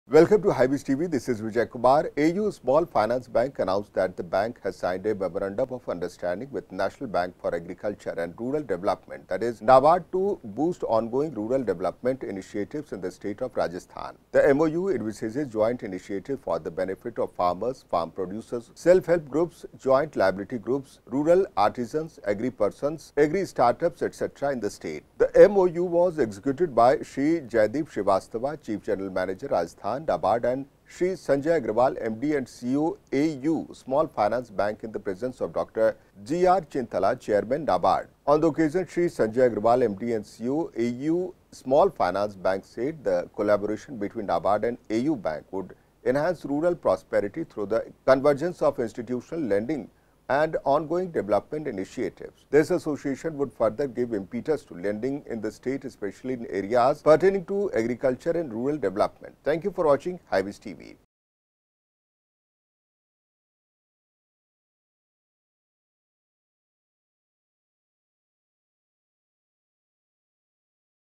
AU Small Finance Bank signs MoU with NABARD || Hybiz tv
AU Small Finance Bank announced that the bank has signed a Memorandum of Understanding (MoU) with National Bank for Agriculture & Rural Development (NABARD) to boost ongoing rural developmental initiatives in the state of Rajasthan.The MoU envisages joint initiatives for the benefit of farmers, farmer producer organisations, Self Help Groups (SHG), Joint Liability Groups (JLG), rural artisans, agri-preneurs, agri start-ups, etc., in the State.The MOU was executed by Shri. Jaideep Srivastava, Chief General Manager- Rajasthan, NABARD and Shri. Sanjay Agarwal, MD and CEO, AU Small Finance Bank in the presence of Dr. G.R.Chintala, Chairman, NABARD.On the occasion, Shri. Sanjay Agarwal, MD and CEO, AU Small Finance Bank said, “The collaboration between NABARD and AU Bank would enhance rural prosperity through the convergence of institutional lending & ongoing development initiatives. This association would further give impetus to lending in the state especially in areas pertaining to agriculture and rural development.”